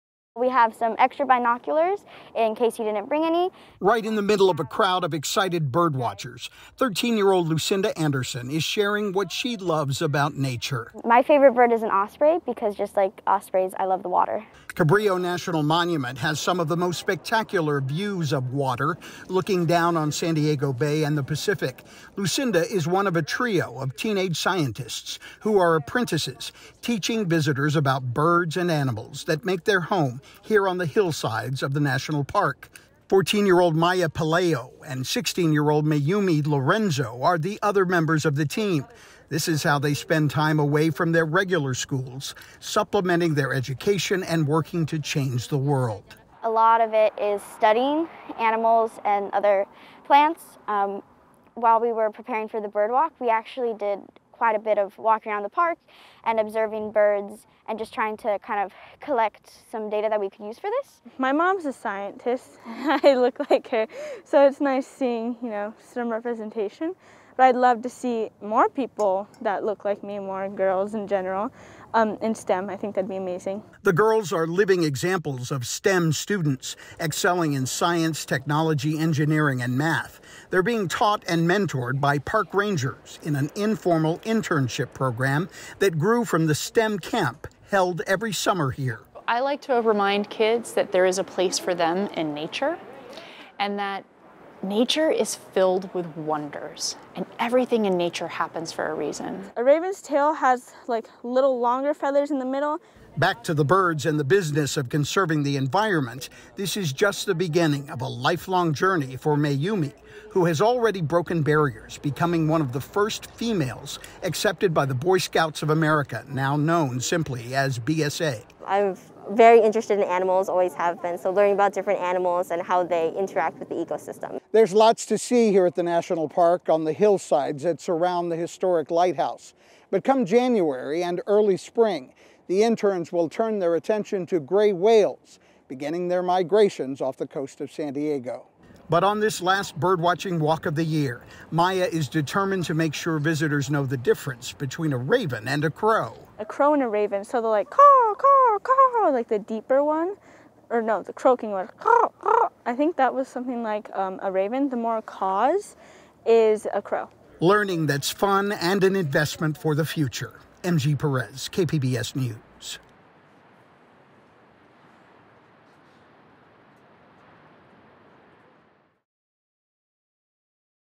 Teenage STEM students lead nature education at Cabrillo Monument
As the sun set on the Cabrillo Monument, tonight, visitors to the national park enjoyed one last special bird walk guided by three young female scientists. They are teenagers with a love for the environment and ambitions to change the world. KPBS Education Reporter M.G. Perez shows us learning that is happening outdoors on a Point Loma hillside.